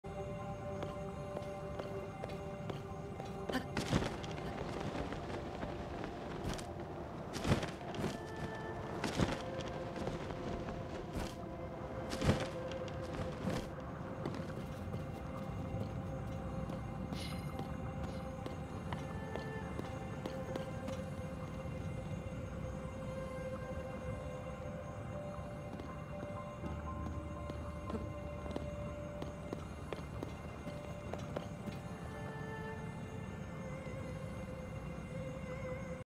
Light Dragon Helm v2.0
I worked on the design more for my light dragon helmet. I like this design a lot more, only wish I could find an alternative to the stakes around her horns as those help keep the build in place from moving.